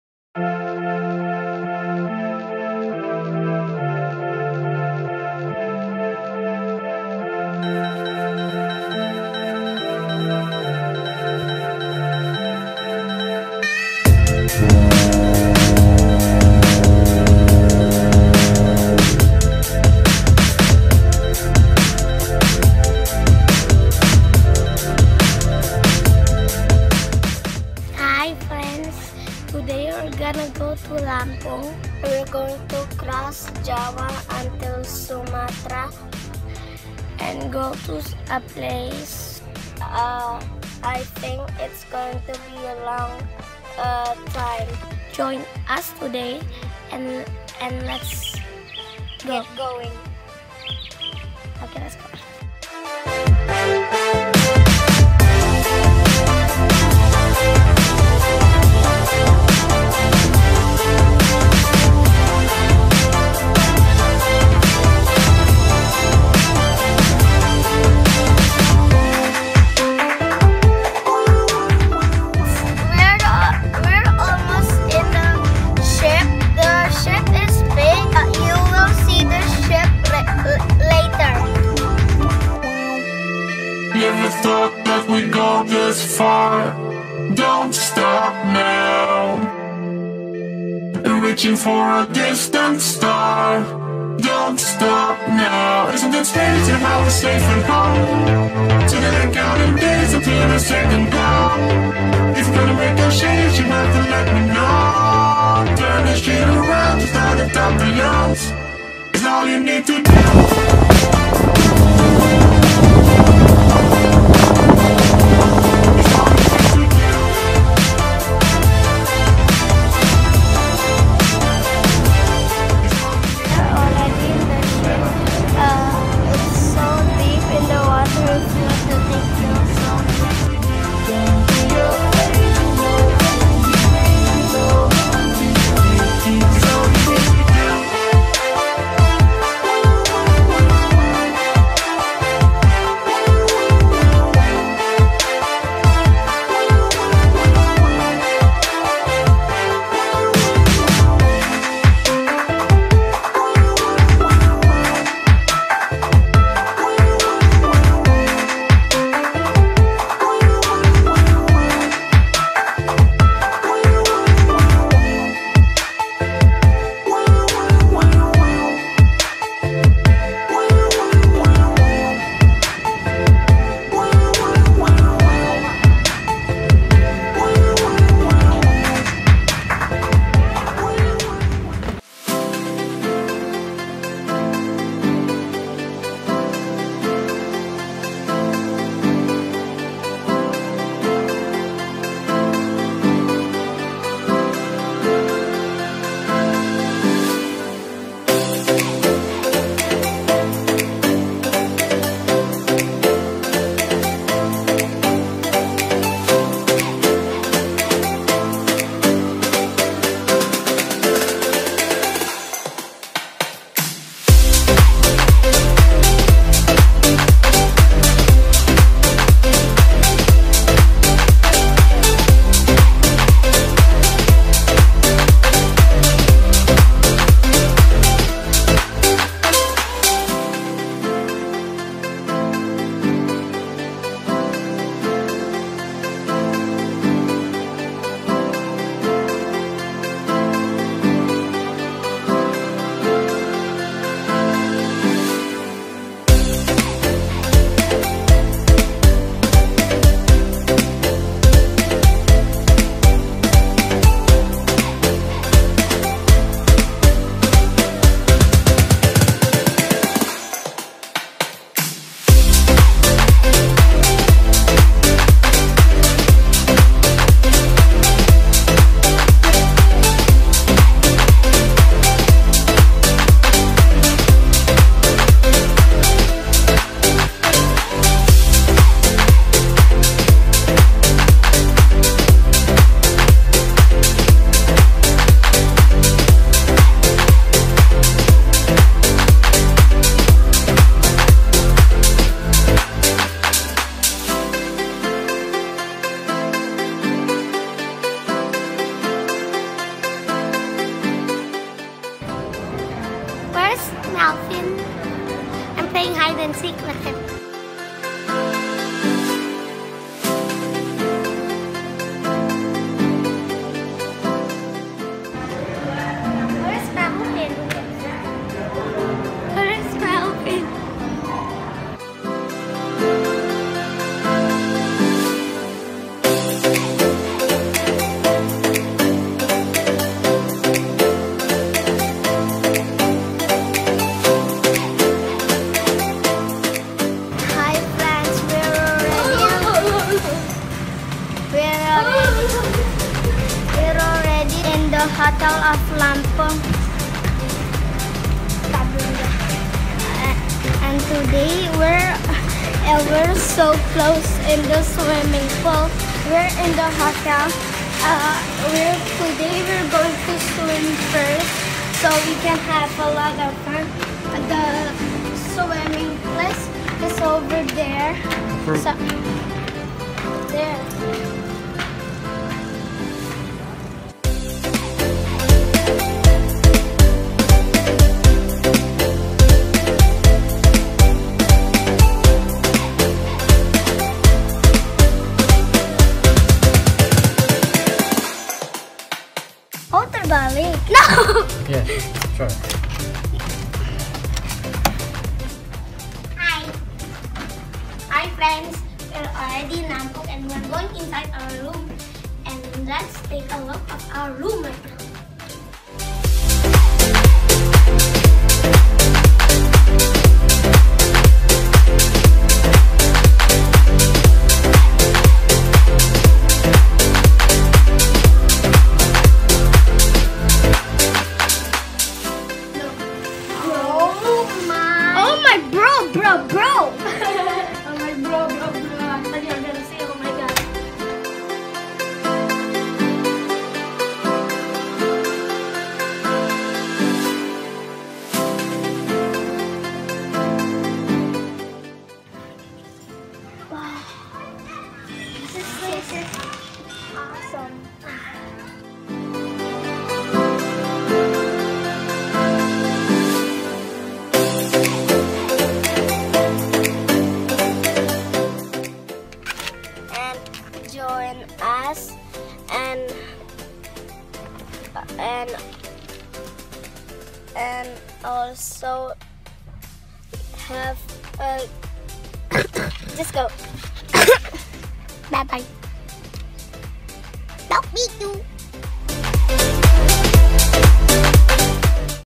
Penyebrangan Merak Bakauheni | Kapal Ferry ke Lampung | Liburan ke Lampung
Mycah Melvyn goes to Lampung
Hari ke-1

Pertama kalinya kita ke Lampung, ternyata banyak tempat wisata yang bagus... sayang kurang di rawat

Hope you enjoy our channel